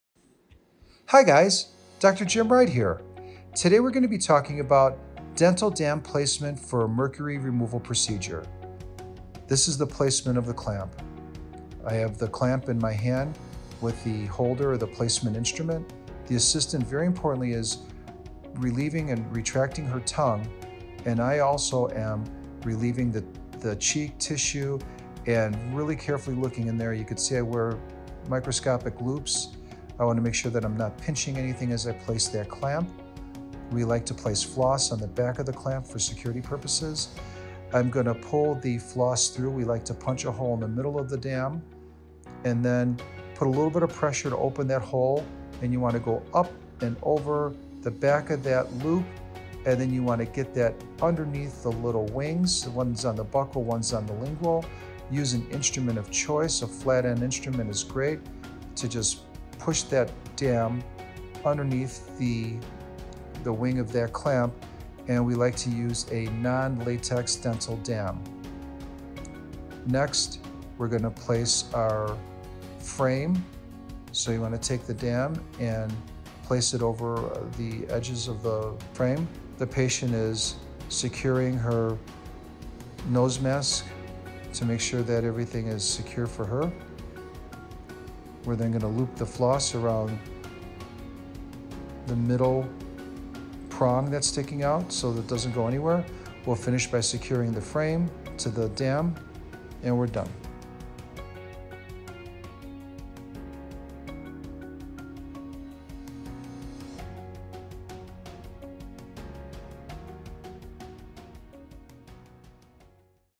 Dental Dam Instructional Demonstration Video by Dr Jim Wright, DDS, Las Vegas Dentist
Dr Jim Wright of Four Seasons Dental Spa demonstrates the installation of a Dental Dam prior to performing a Mercury Removal procedure in their Las Vegas, Nevada dental office. This video shows Dr Wright fitting the dental dam with his dental assistant on a live patient. Please follow our channel for more educational and instructional information about dentistry and healthy oral care.